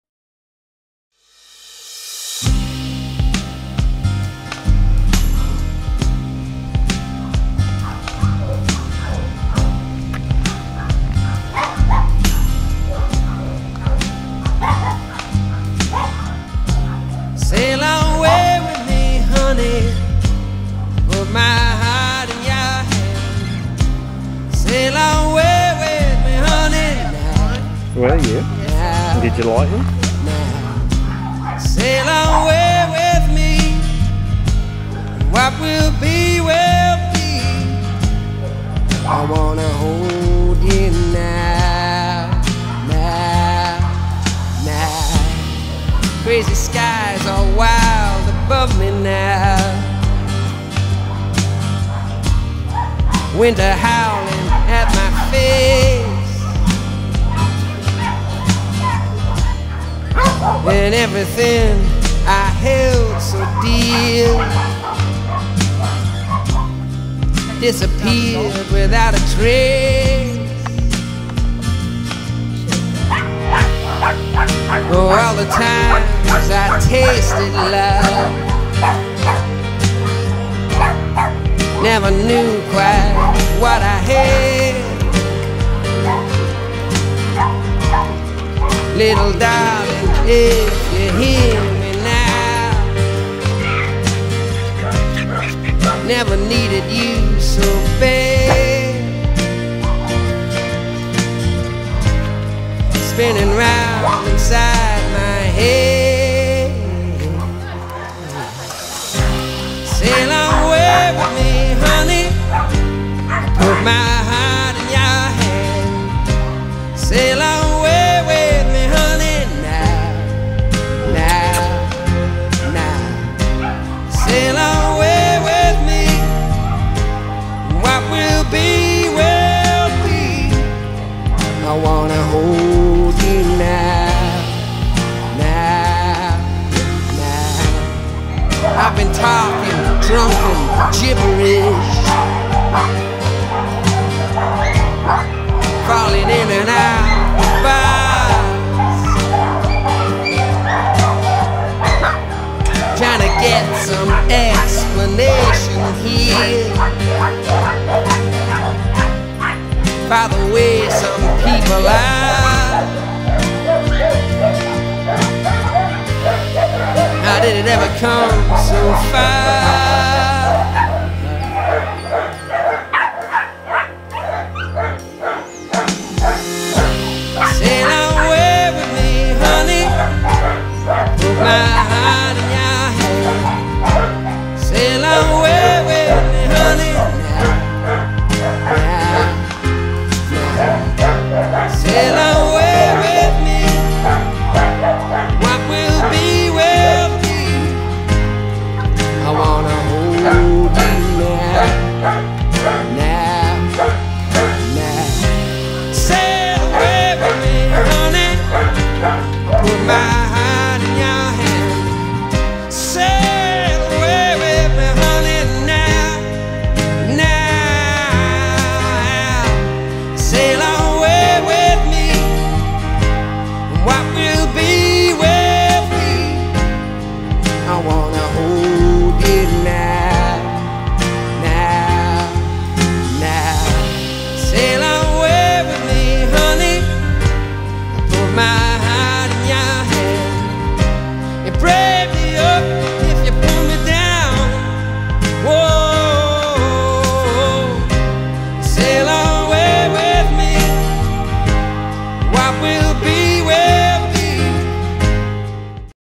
Monika's Doggie Rescue - Dog Adoptions (A day at the shelter)
This video was compiled from a day @ the shelter.